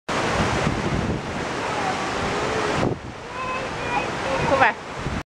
Wat een wind!
Geen zacht briesje, maar een wind van heb ik jou daar.
Stoelen vlogen over de terrassen en overal afgeknakte palmtakken.
Gelukkig niet de hele week.
Tunesië, mei 2013.